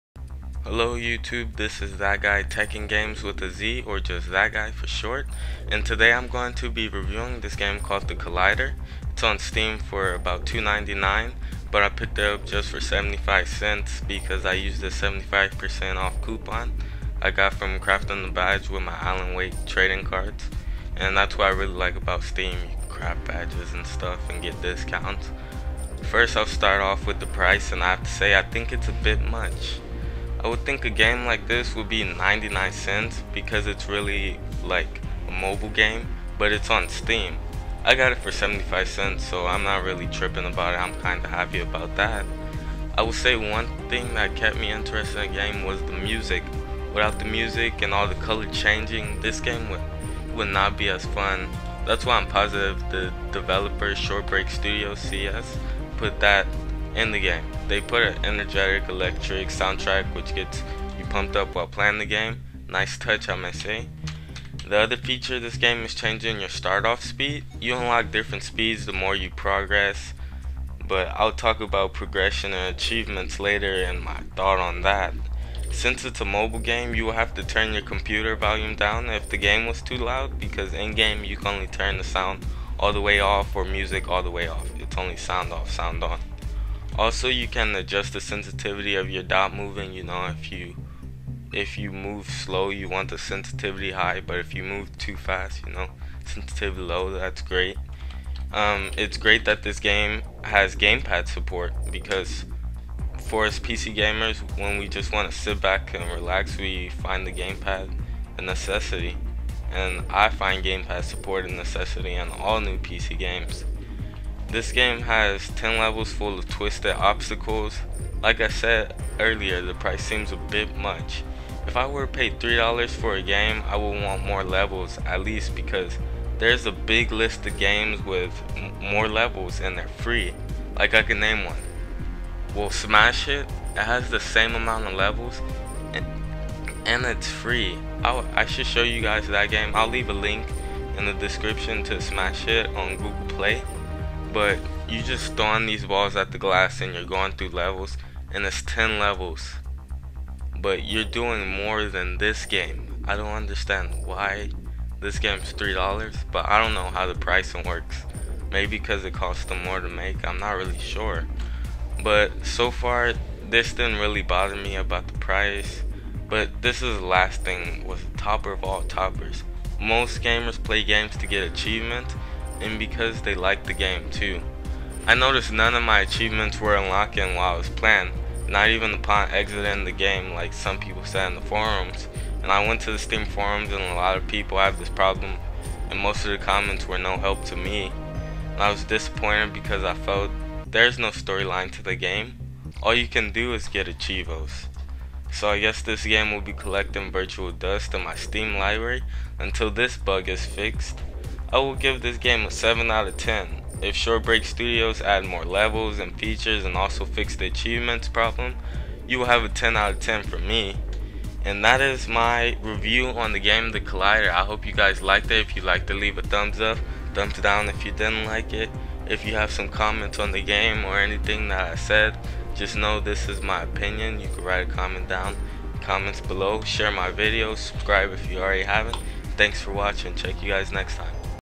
"The Collider Review"
"The Collider is a fast-paced and compelling arcade game that will put your reflexes to the test."

I gave this game a 7/10 in the video, but my new rating is 8/10 because I reinstalled the game and then my achievements came up.

I actually already got all the achievos!

I would like to know what you think of the game?